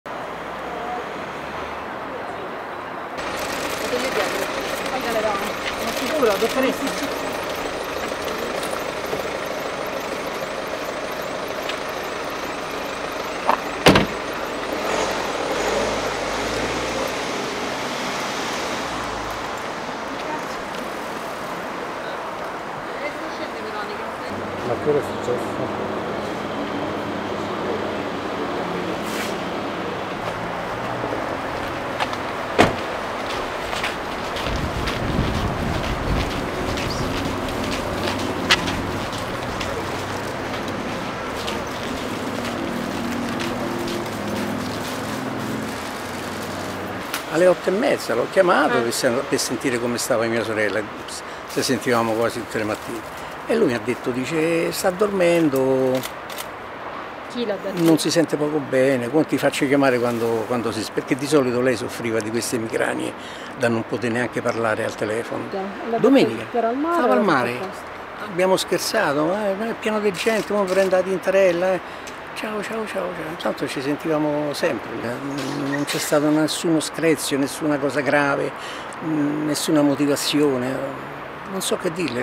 Uccide la moglie a colpi di pistola, il fratello della vittima: «Ho sentito mio cognato al telefono»
Una donna di 72 anni è stata uccisa dal marito di 76 a colpi di pistola in un condominio di via Mascagni, a Roma. Il fratello della vittima racconta: "Avevo sentito mio cognato stamattina al telefono, mi ha detto che mia sorella si sentiva poco bene".